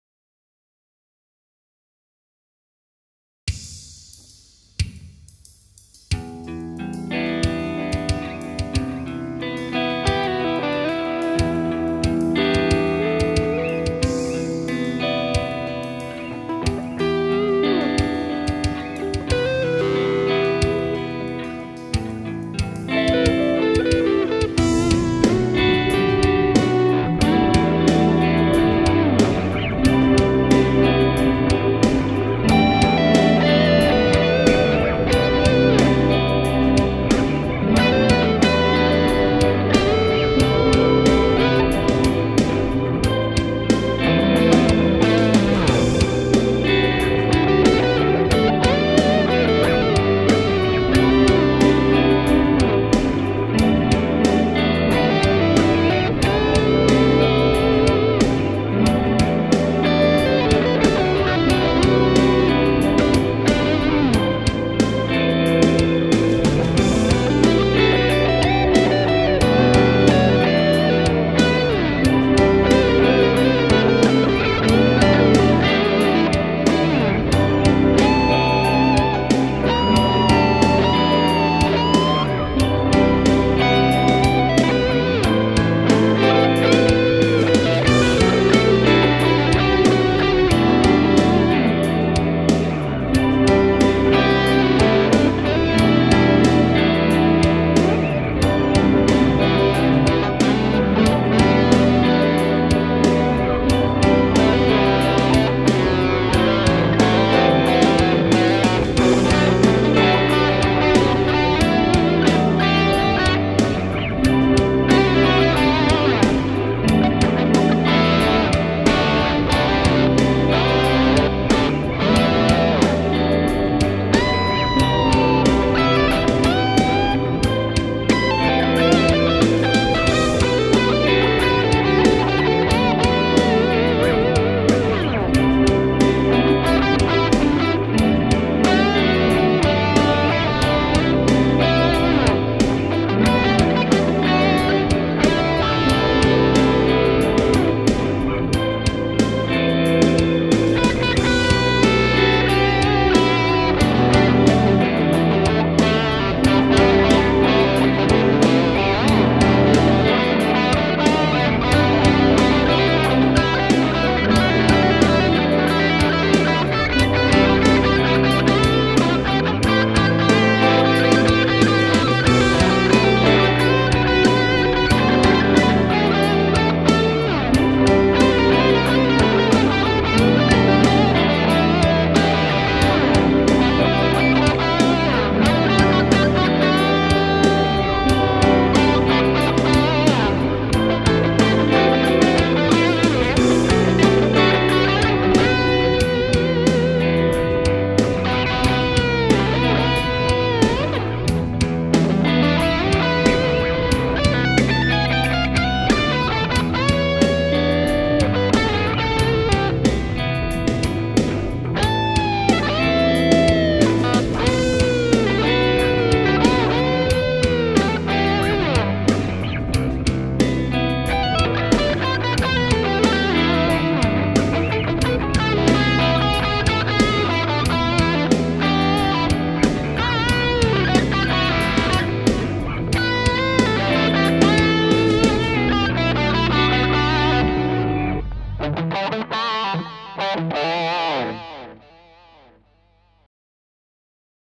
Sunday Jam
Messing about with some improv after a big lunch.
Chops are a bit sloppy.